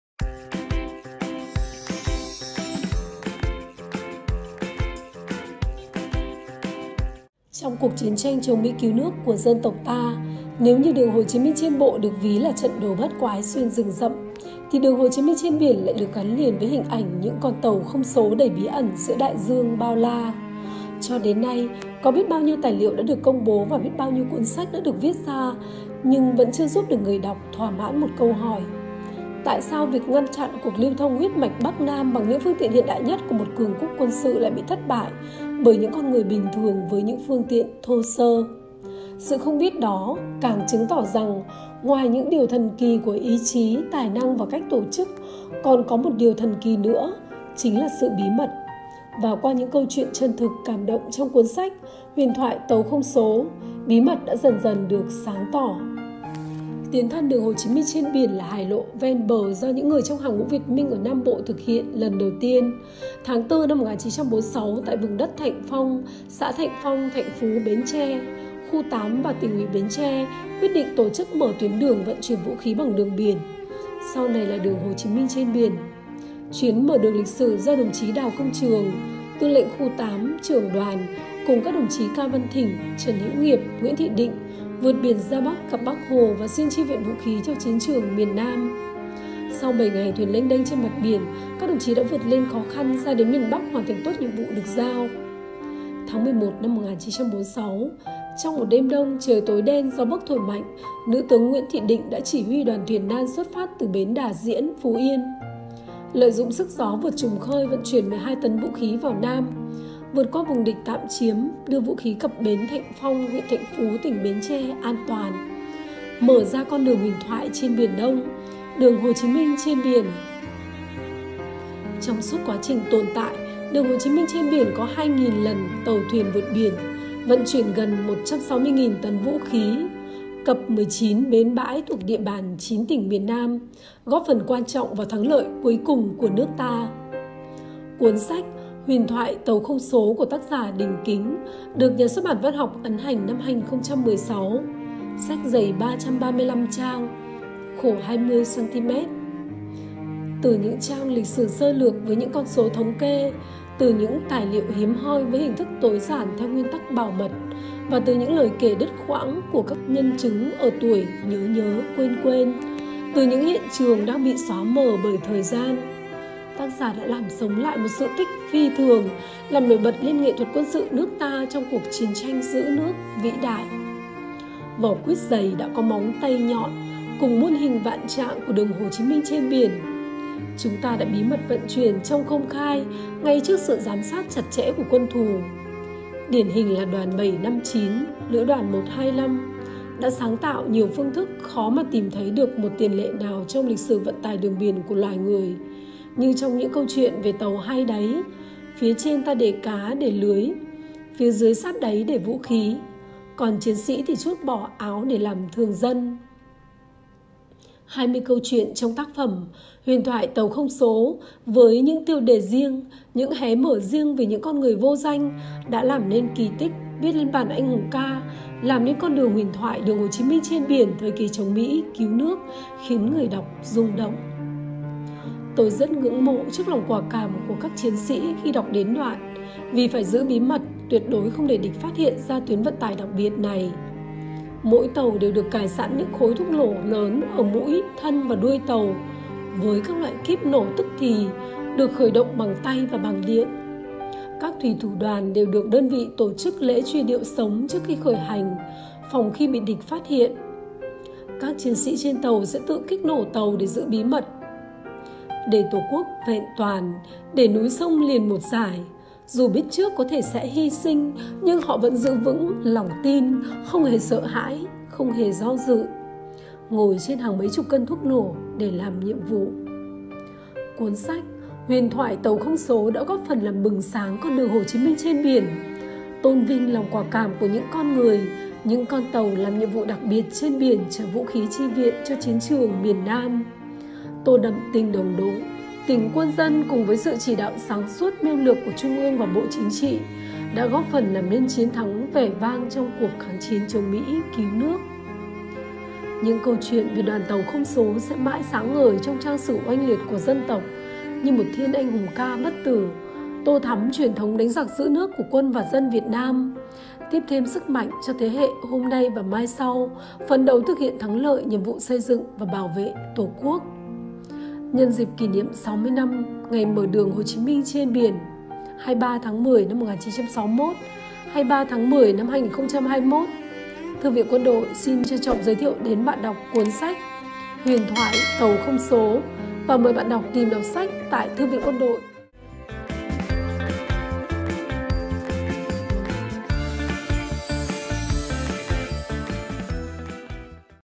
Huyền thoại Tàu Không Số
Giới thiệu tác phẩm: Huyền thoại Tàu Không Số
Thực hiện: Thư viện Quân Đội - Bộ Quốc phòng